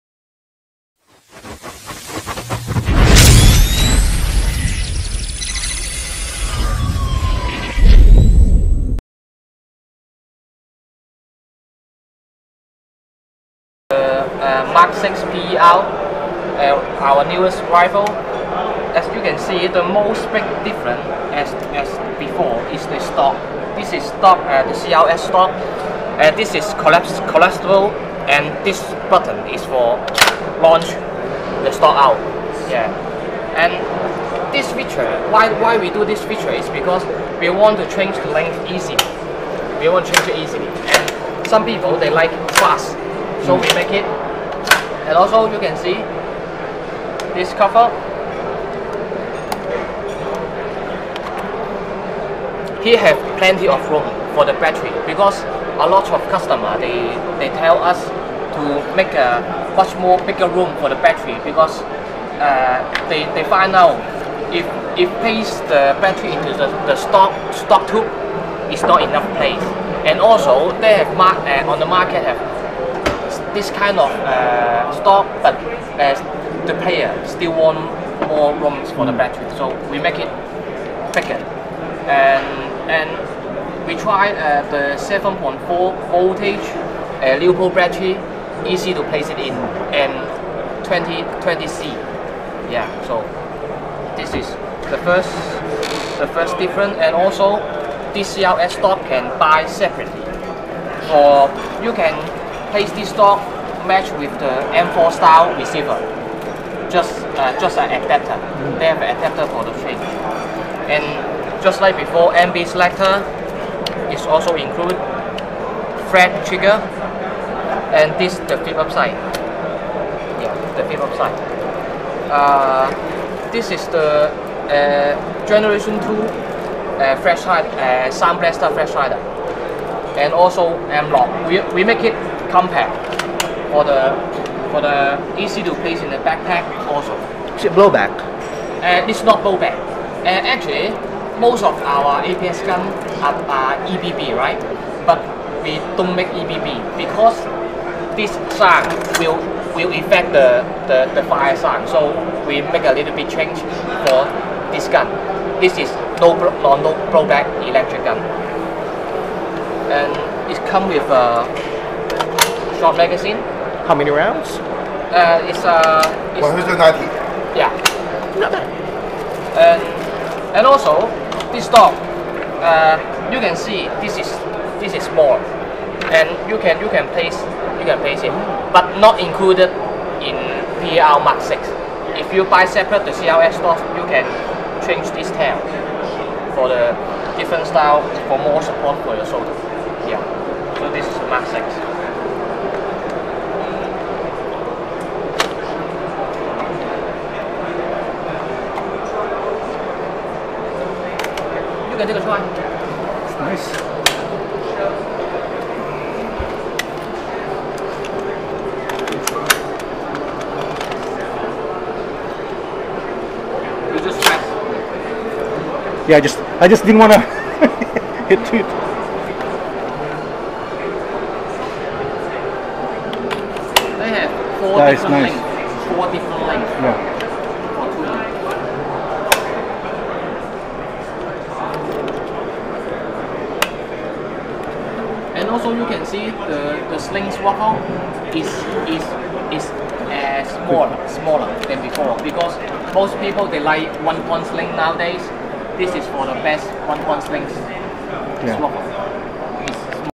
IWA Outdoor Classics 2018: APS PER MKVI AEG
Joe Lau talks about the new APS Phantom Extremis Rifle (PER) MKVI which comes with a CRS stock and a sound amplifier flash hider during the IWA Outdoor Classics 2018 in Nuremberg, Germany.